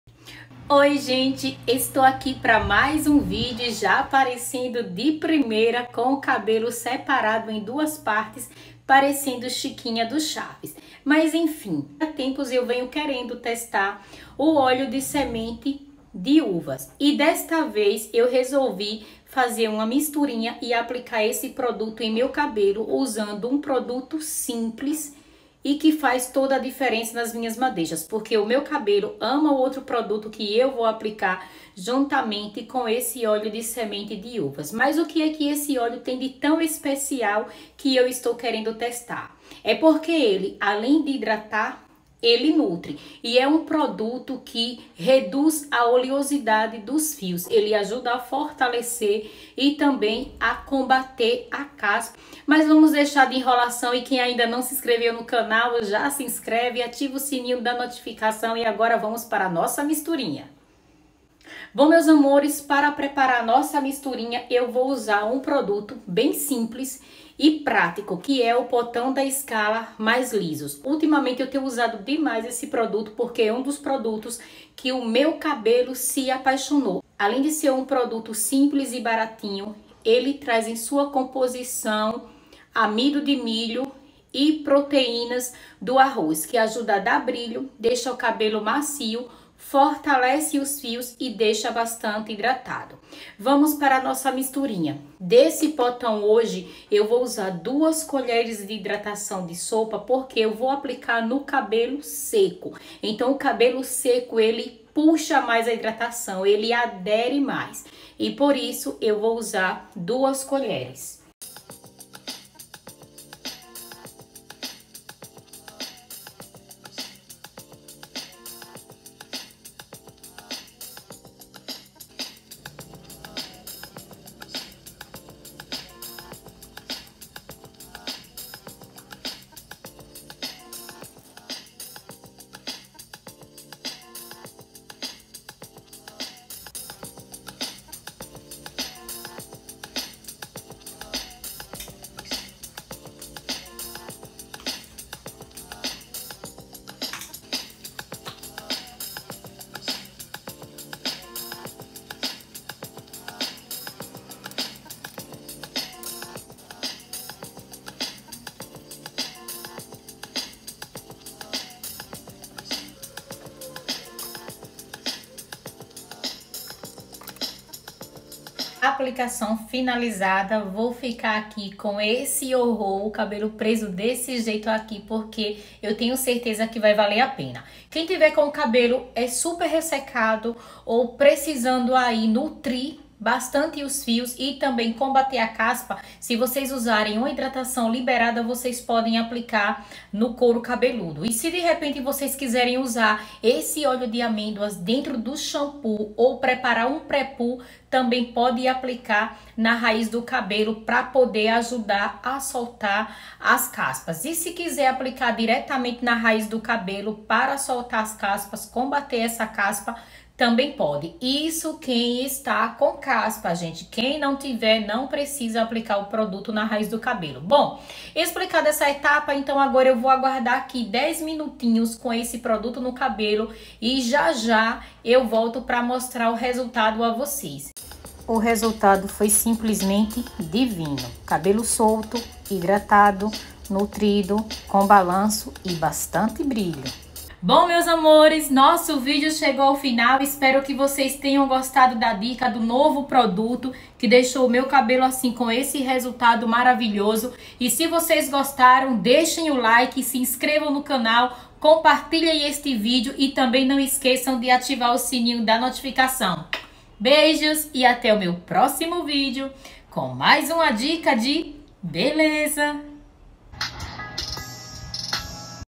CABELO PERFEITO COM APENAS 2 PRODUTOS/ Silvania Sends
Vídeo extra desta semana para vocês suas lindezas.
Dois produtos baratinhos que deixaram meu cabelo perfeito.
Apliquei no cabelo seco, duas colheres de sopa de hidratação do potao da Skala mais lisos e uma colher de sopa do óleo de semente de uva.
A misturinha perfeita para um cabelo perfeito.